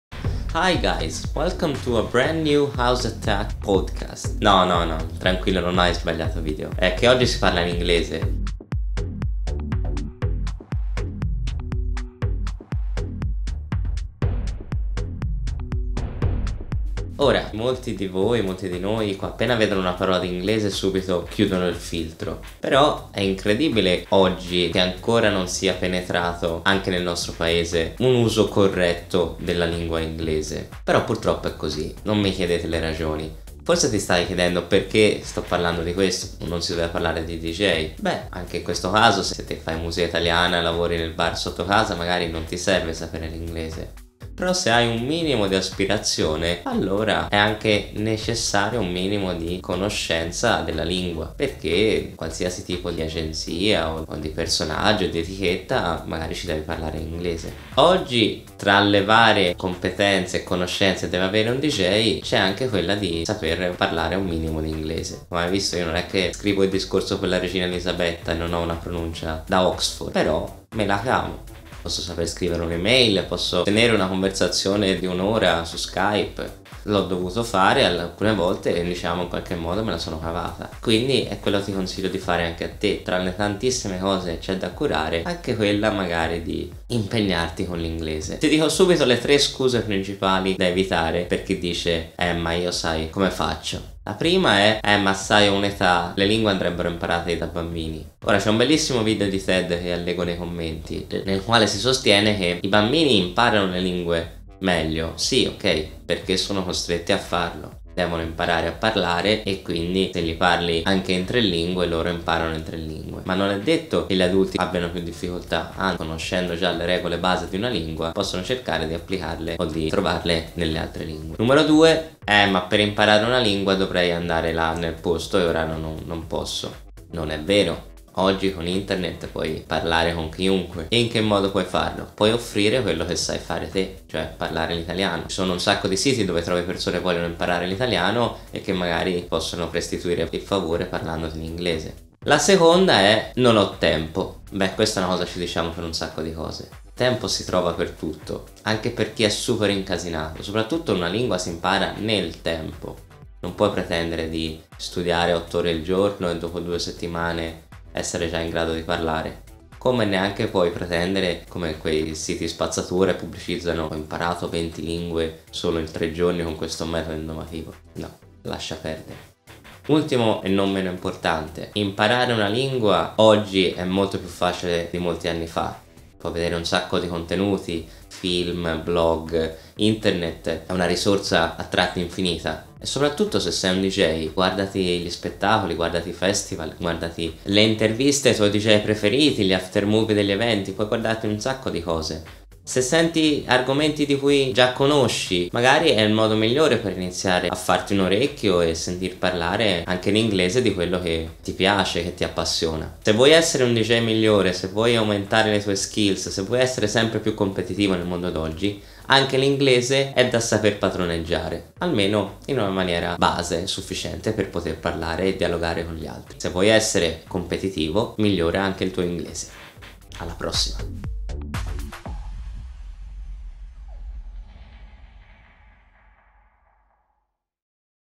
I DJ e l'Inglese. Essere DJ Oggi #15
L'inglese è fondamentale, oggi ancora più di prima, per chiunque voglia riuscire ad espandere il proprio raggio d'azione, a maggior ragione per un DJ. Ciò non significa che si debba essere dei maestri, ma almeno padroneggiare la lingua ad un livello base. Con gli strumenti a disposizione grazie ad internet è ancora più facile e diventa più fattibile se inizi ad ascoltare contenuti in lingua originale, magari interviste, programmi, video degli argomenti che già conosci nella tua lingua.

Versione restaurata del video con sigla, editing, copertina, sottotitoli e musica di sottofondo.

Nuovi Video